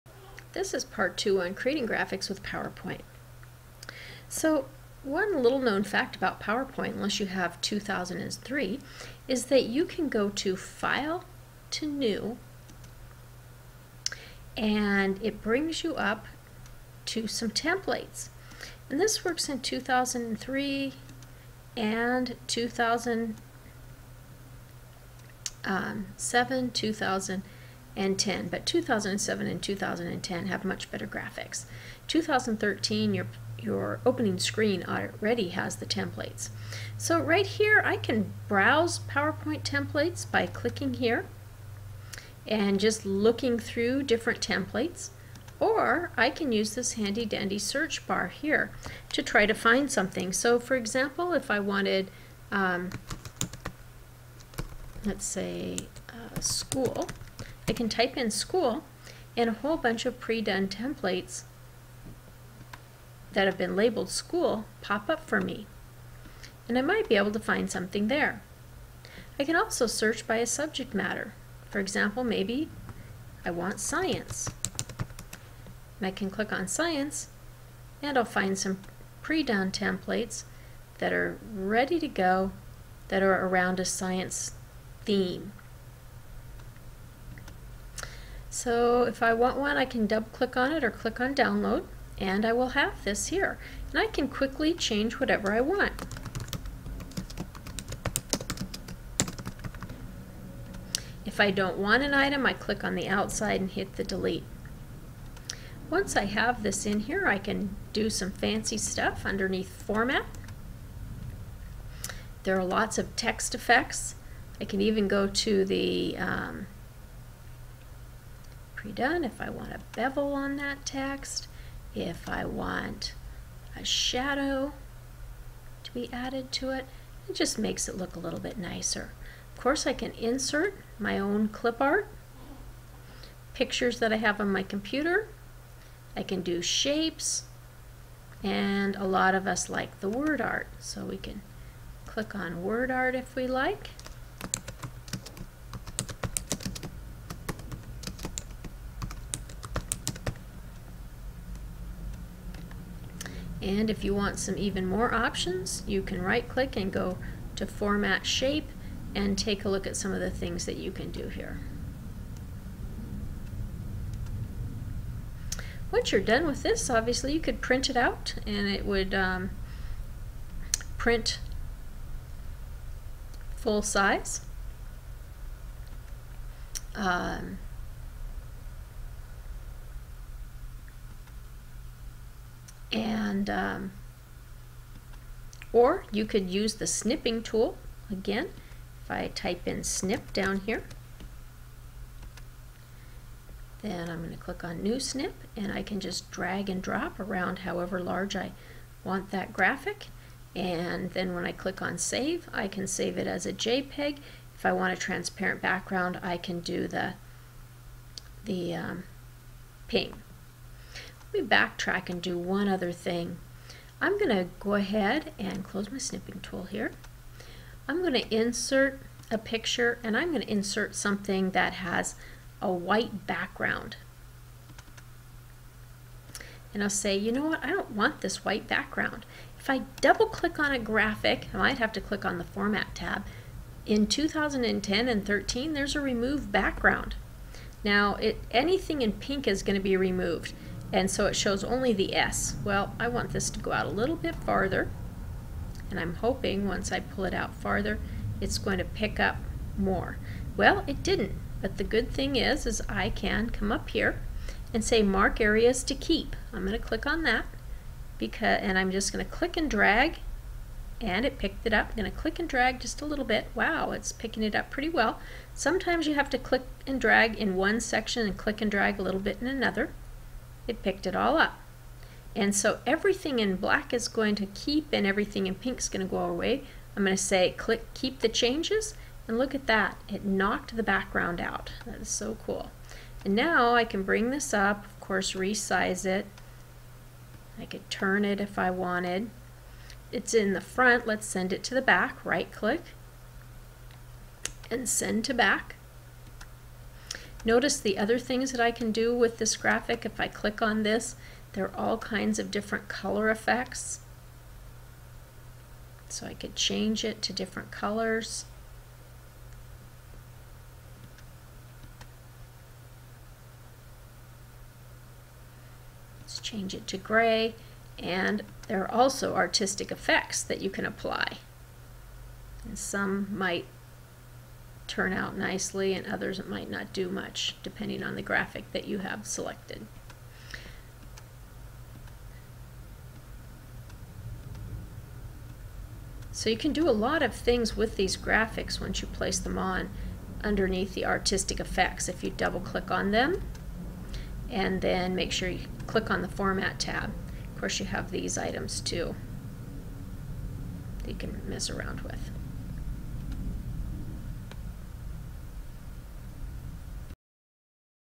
Creating Graphics with Power Point Part 2
PowerPoint can create awesome graphics without the learning curve of Photoshop or other graphics editor. In this video, you will learn how to use predone templates to create a snazzy graphic in a hurry.